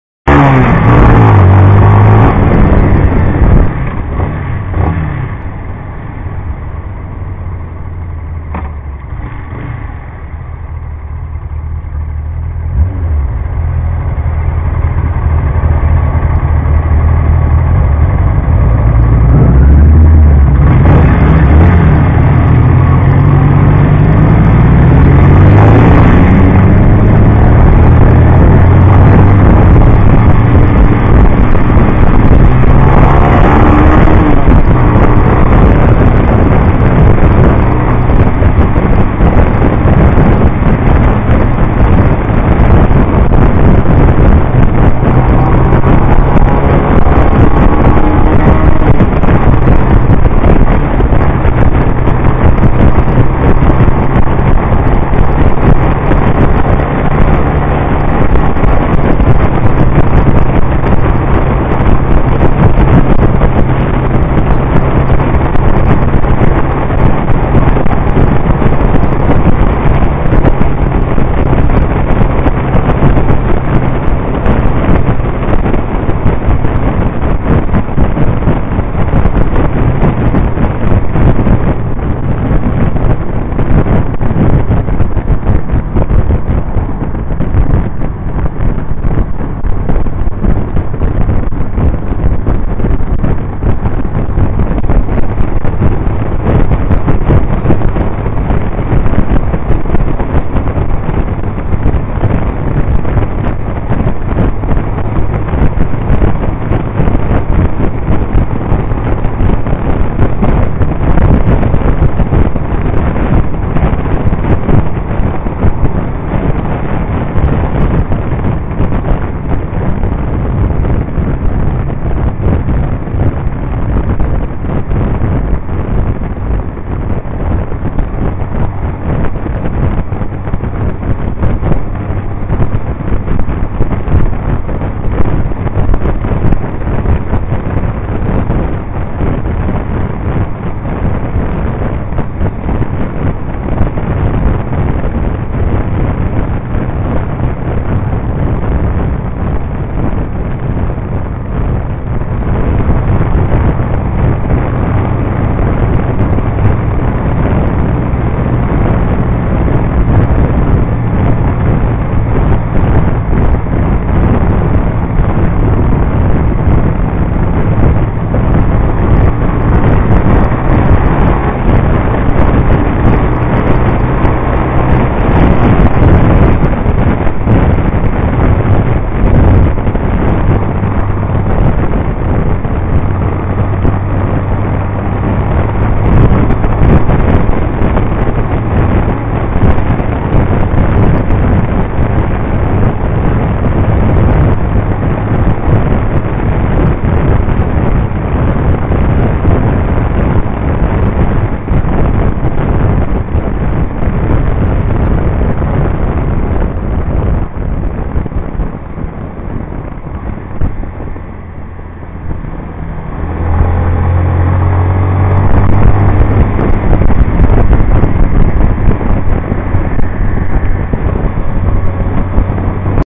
Onboard a CBR125 through Bristol NB
In this video we have a 1100 Honda Interstate and a 750 Suzuki Katana.